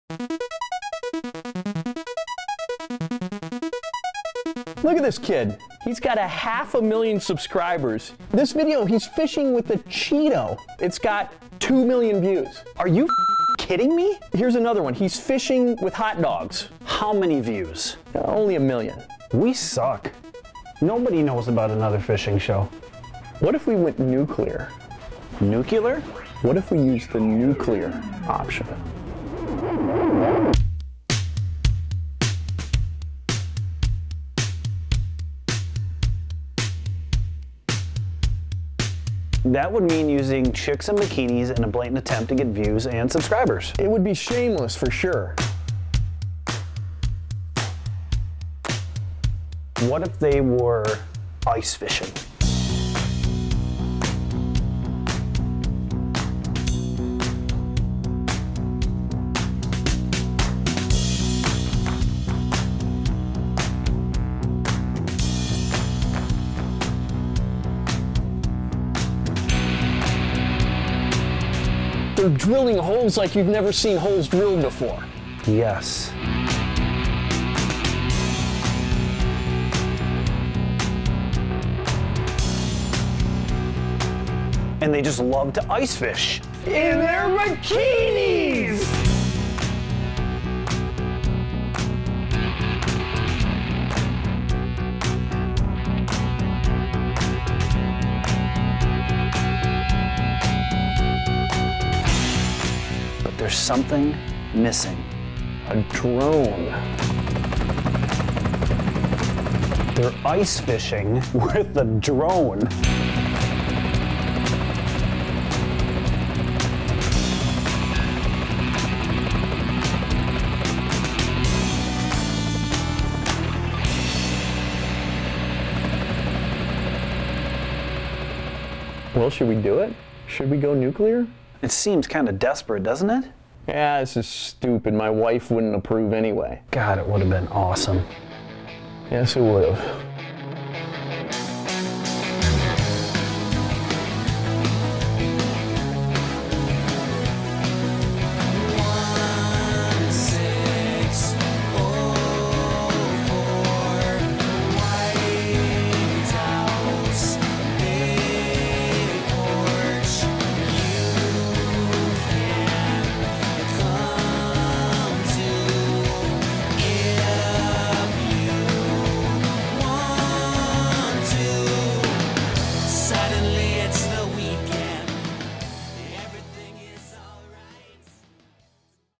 Ice Fishing Dream Team!
We called an emergency meeting to brainstorm ways to gain a foothold on YouTube.  Girls in bikinis ice fishing with a drone is a good idea, right?

Special thanks to North Star Aerial.